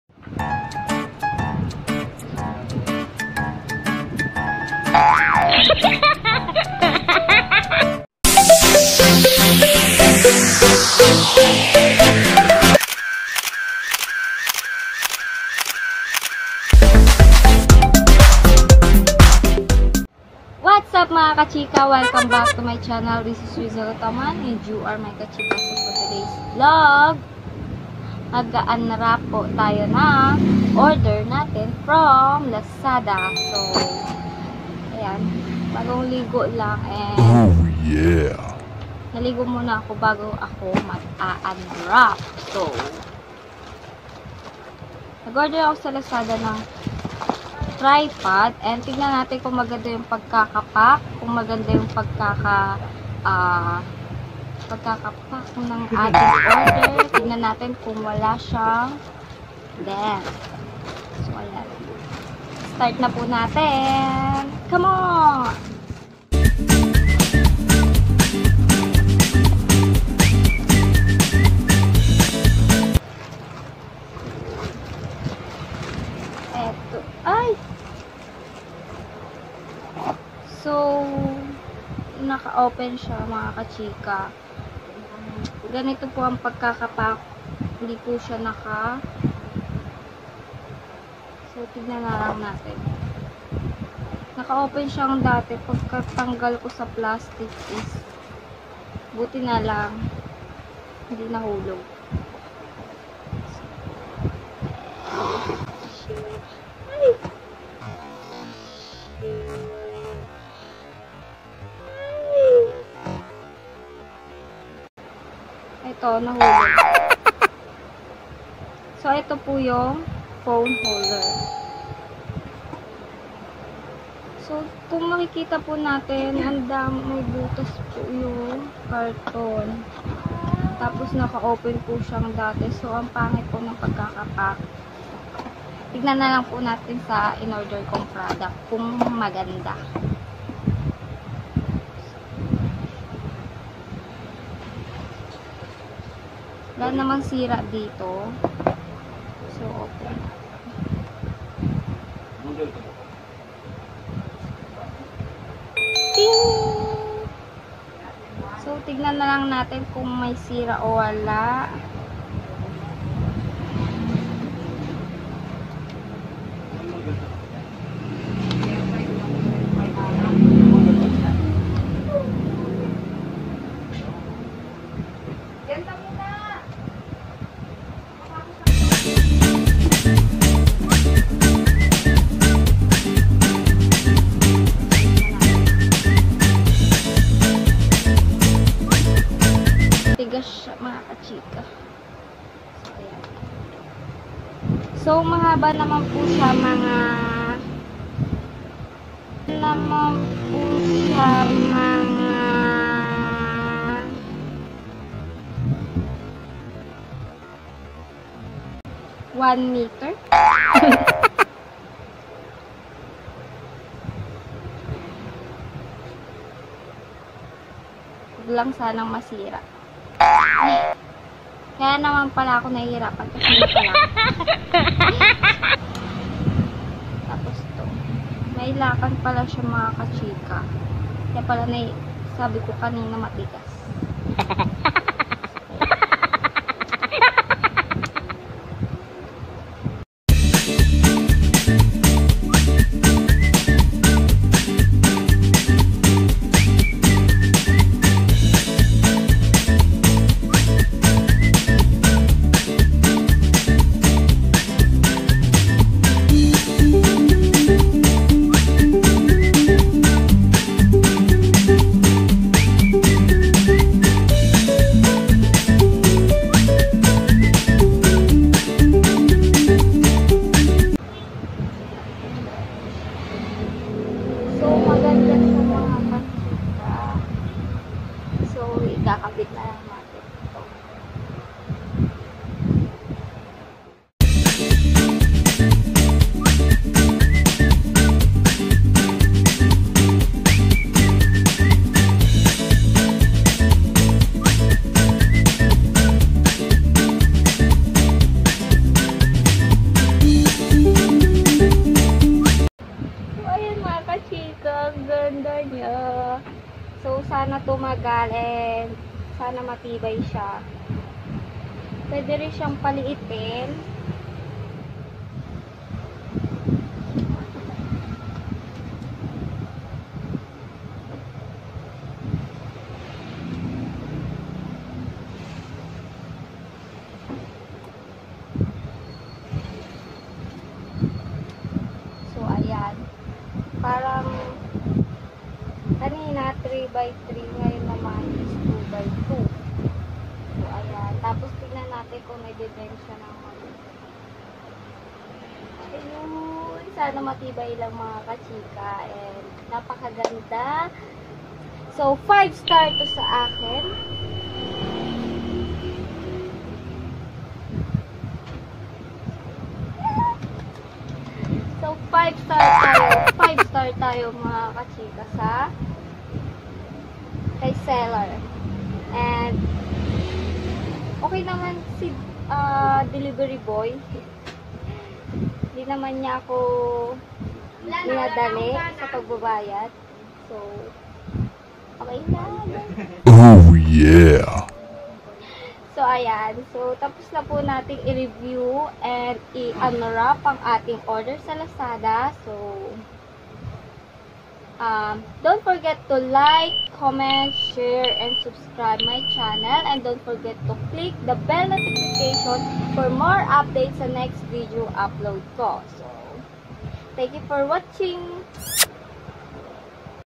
Unbox My 1st Tripod | Rizza Gotoman | I Like It
I really Like it when I was doing unwrap, especially with my 1st Tripod.